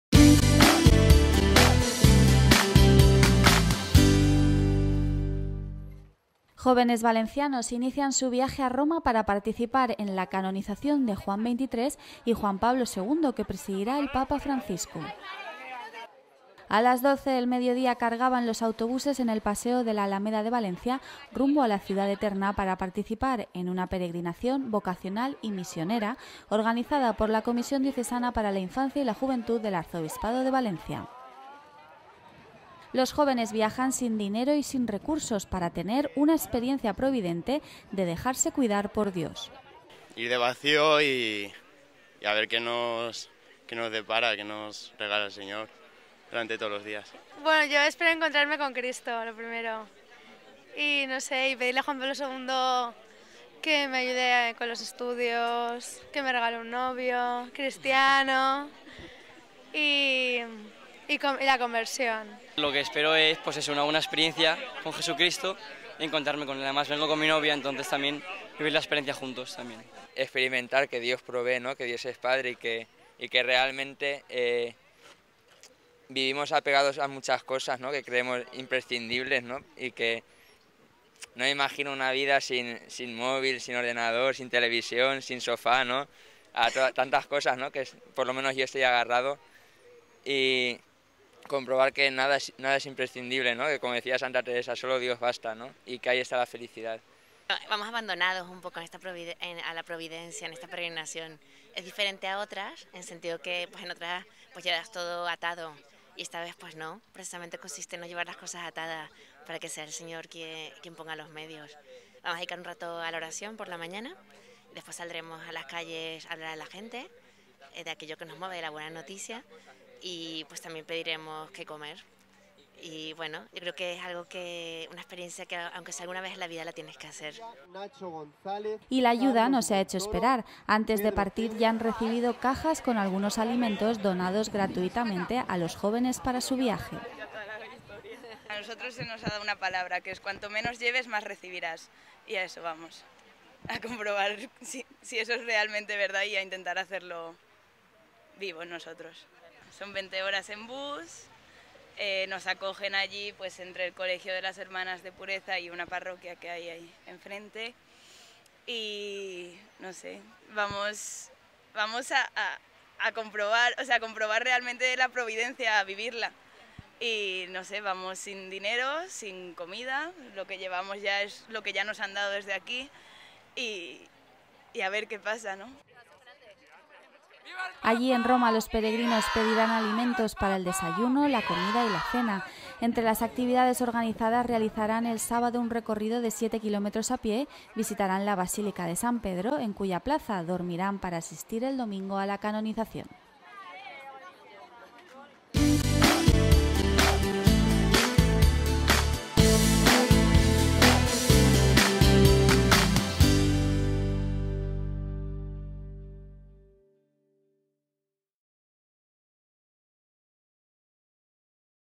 Salida jóvenes de Valencia a canonizaciones en Roma
Salida jóvenes de Valencia a canonizaciones en Roma. Servicio Audiovisual Diocesano